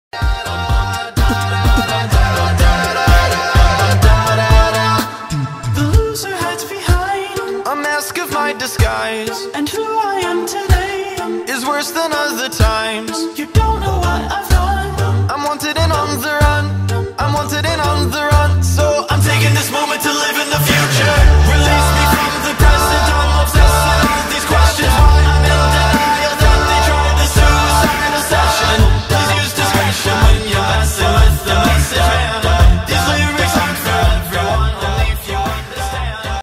Masked Messenger [Meme]
Just figured out how to use chroma key compositing, so I decided to test it out with a new meme! I think it worked out really well, but JEEZ those arms killed me to edit.

The character's name is Kanko, and he belongs to me. He's such a sad possessed boy ;-; he needs love.

The song is a Twenty One Pilots Acapella medley by Austin Jones.